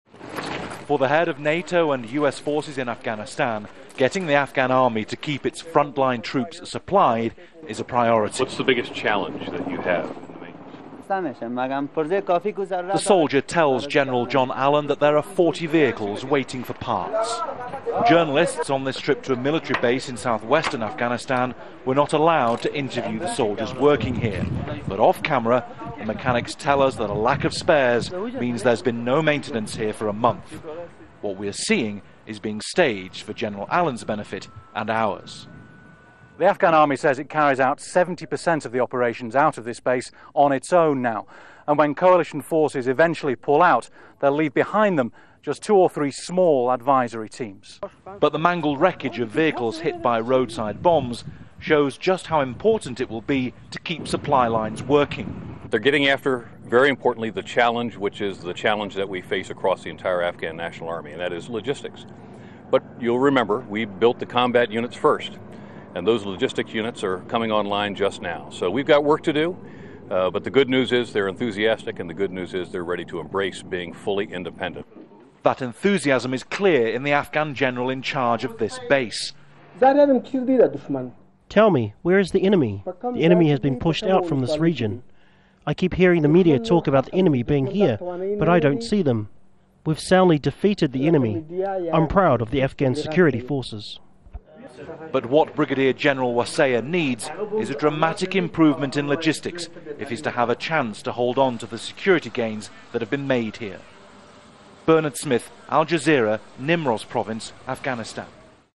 Afghan army prepares to take over country security
One of the main challenges facing Afghanistan's army is the withdrawal of foreign forces from the country.  The Afghan Army says it carries out 70% of the operations out of this base on its own now.  and when coalition forces eventually pull-out from the province, in two to three months, they'll leave behind them just two or three small advisory teams.  NATO commanders are finding it difficult to get the Afghans to create an efficient system to keep the military well  supplied.   Al Jazeera's Bernard Smith reports from Nimroz province.